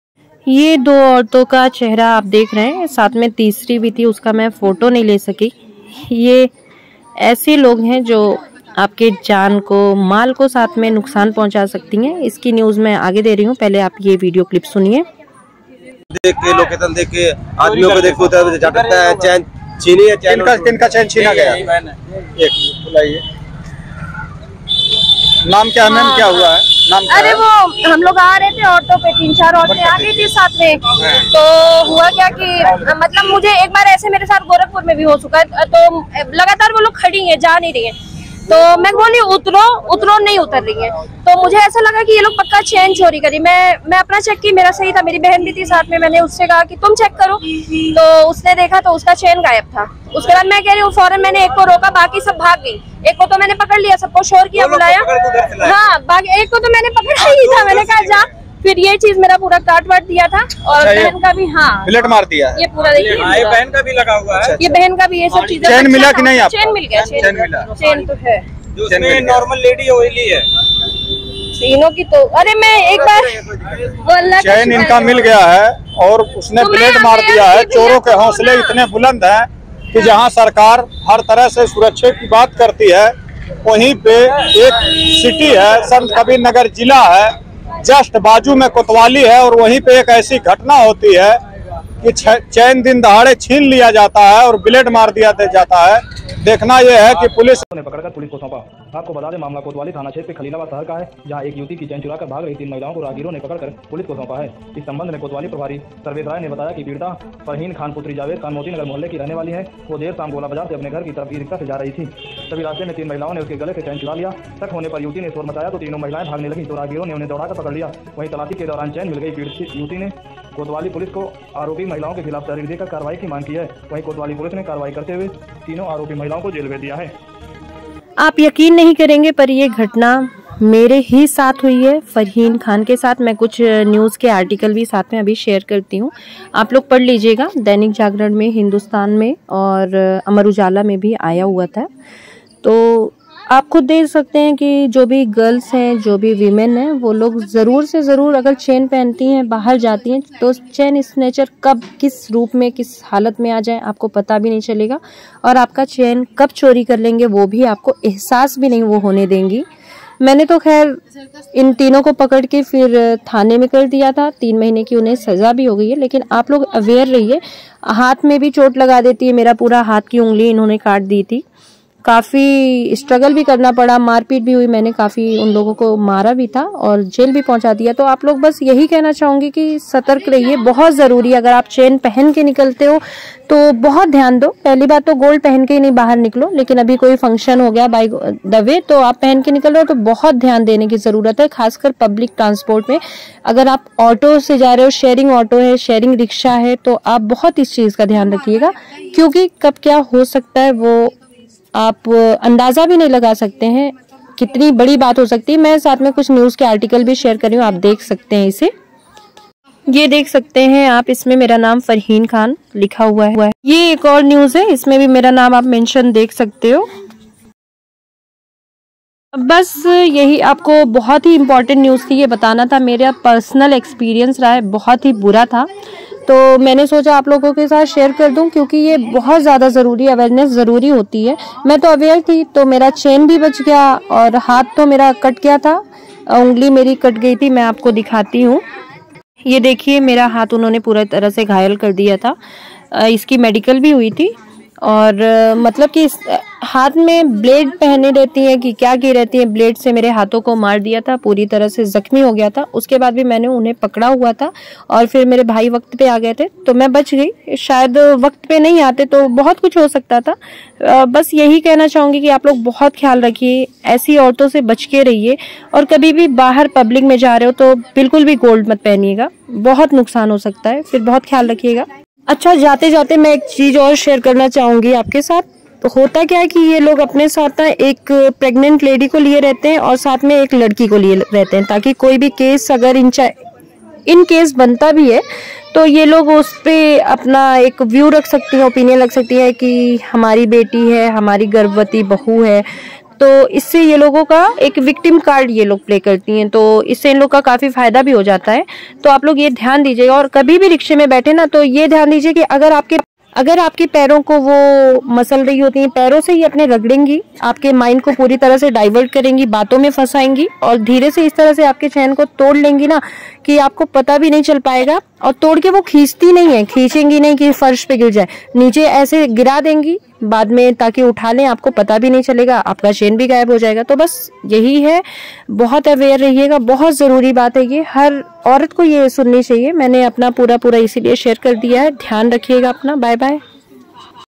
सोना पहन कर मार्केट जाने वालों के लिए important msg🚫🚫 | Farheen Ka Kitchen
अगर आप घर के बाहर सोना पहन के जाती हैं तो इस वीडियो को जरूर देखे 🙏🙏 important msg | Farheen ka kitchen